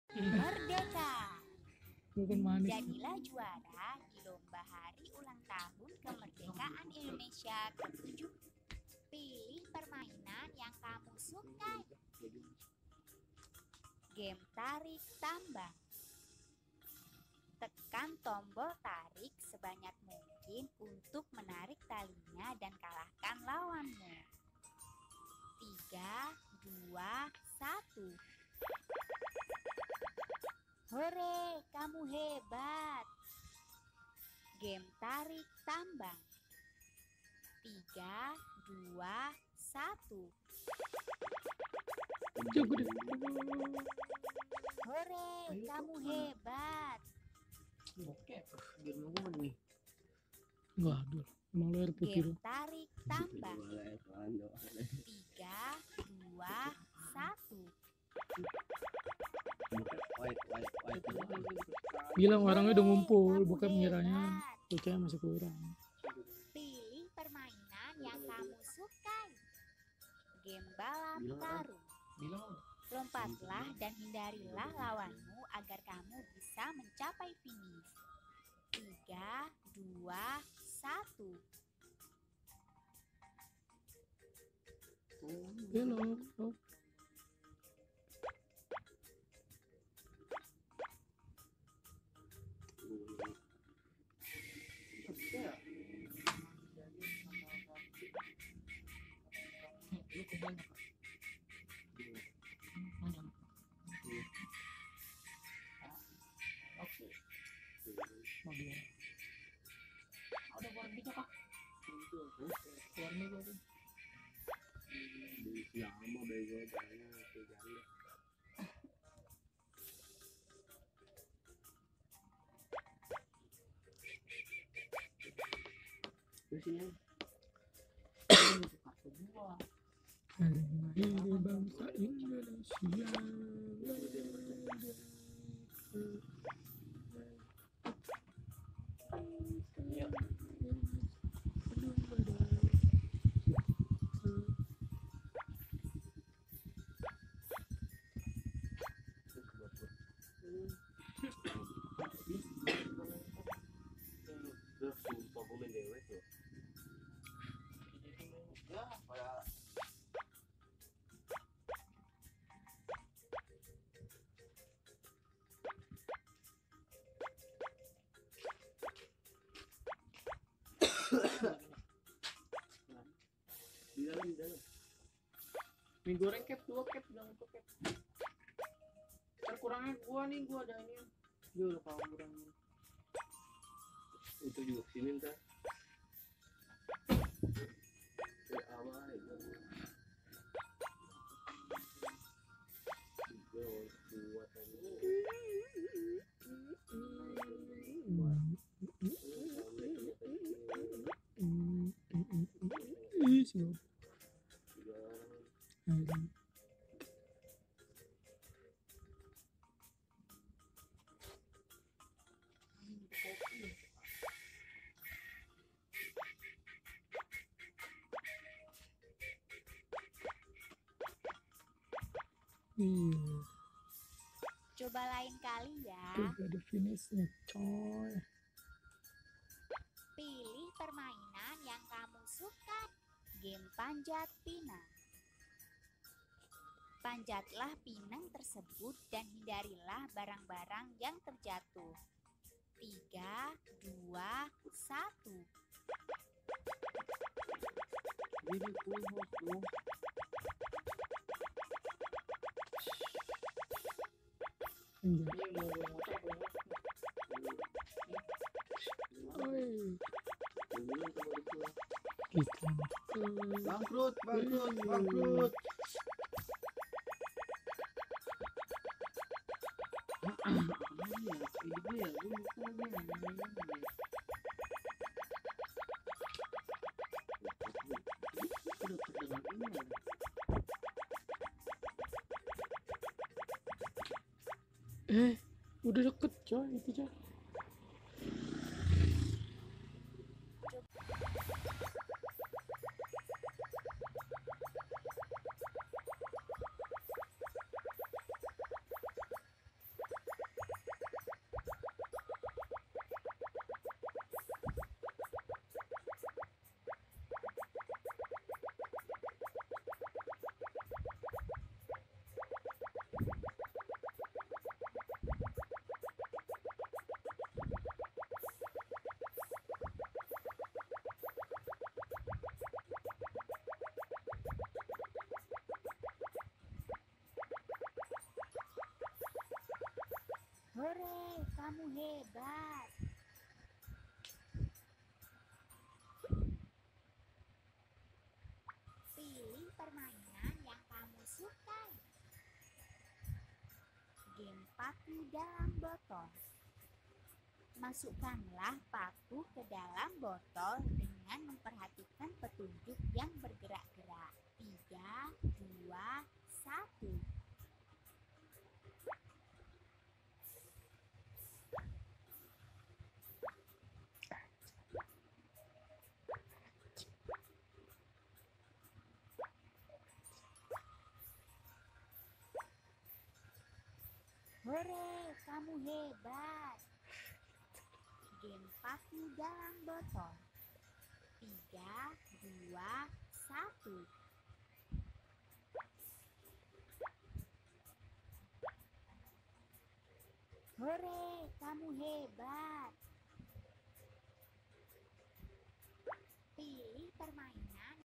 LOMBA 17an DI HAPE!!!! | GAME 17an MERDEKA
Ikutan lomba 17an panjat pinang, balap karung, masukin paku sama tarik tambang di hape